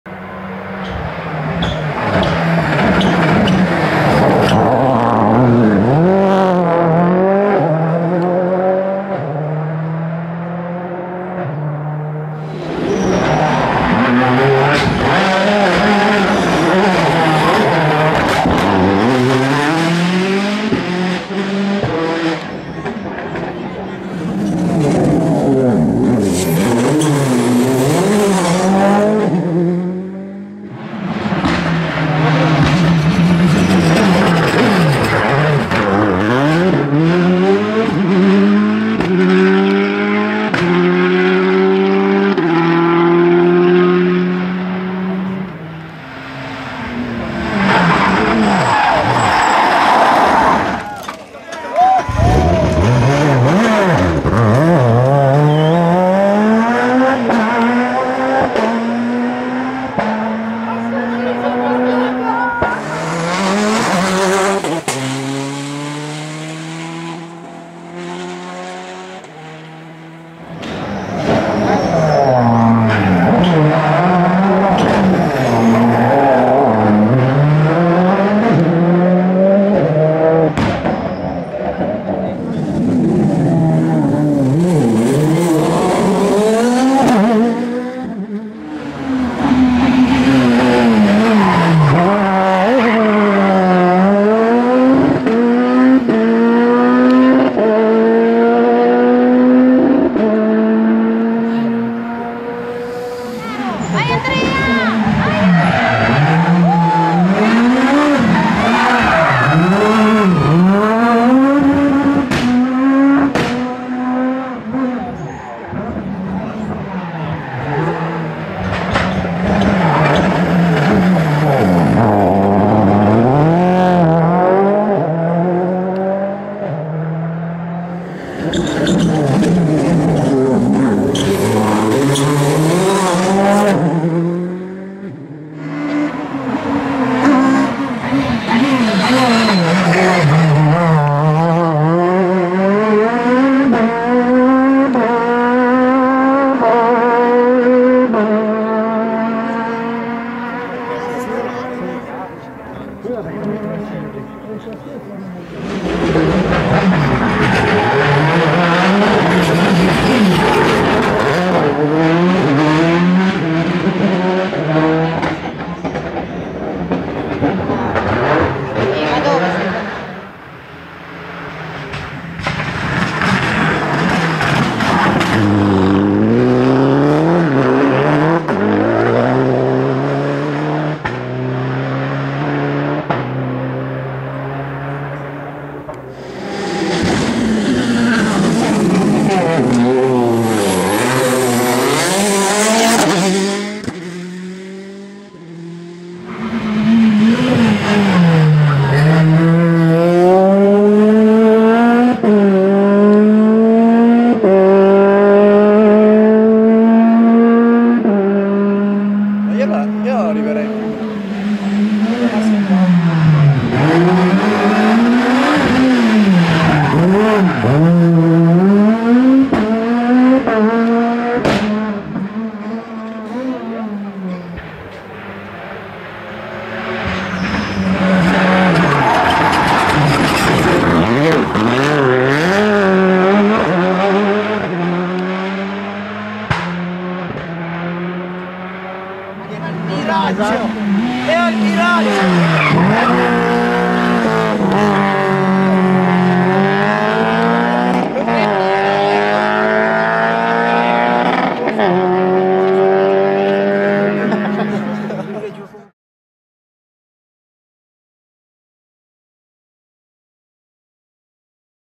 32° Rally Trofeo ACI Como 2013 - Pure Sound & Show [HD]
Event: Rally di Como 2013
Prova Speciale 3 "Val Cavargna" 5 "Alpe Grande" 6 "Argegno-Schignano"
Classifica Finale:
1.Fontana - Arena (Ford Focus WRC)
2.Porro - Brusadelli (Ford Focus WRC)
3.Re A. - Sala (Citroen Xsara WRC)
4.Roncoroni - Vozzo (Peugeot 207 S2000)
5.Diomedi - Turati (Citroen Xsara WRC)
6.Vellani - Lamonato (Peugeot 207 S2000)
7.Vittalini - Tavecchio (Citroen DS3 R3T)
8.Puricelli - Falzone (Ford Fiesta WRC)